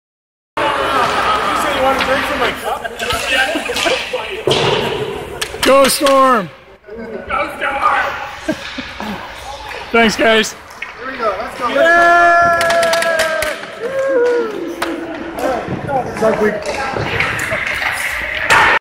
ARL team photos - Storm 2015
Some teams just have MORE FUN than the rest of us! A tribute to Scott Chadbolt and his friends on team Storm.